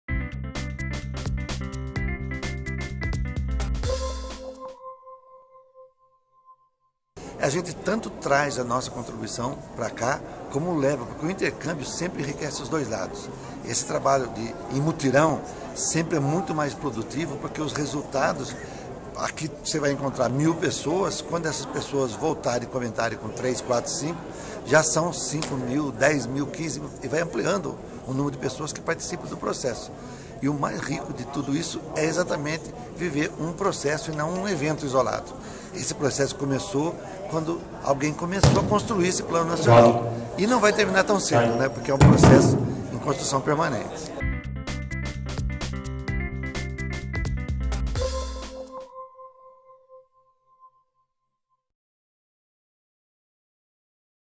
Intercâmbio de propostas e opiniões enriquece a construção de políticas públicas
3ª CNC - 29.11.13: Até domingo (1º), a III Conferência Nacional de Cultura (3ª CNC) vai debater políticas públicas para o setor cultural e discutir o Sistema Nacional de Cultura. Divididos em quatro eixos temáticos, cerca de 2 mil pessoas - delegados, convidados e observadores - vão debater e escolher as propostas que serão votadas na plenária final do evento. A CNC estabelecerá 64 diretrizes (20 prioritárias) para ser seguidas pelos gestores culturais. Segundo o observador Alberto Capucci, o intercâmbio de propostas e opiniões que é feito na conferência enriquece a construção de políticas públicas.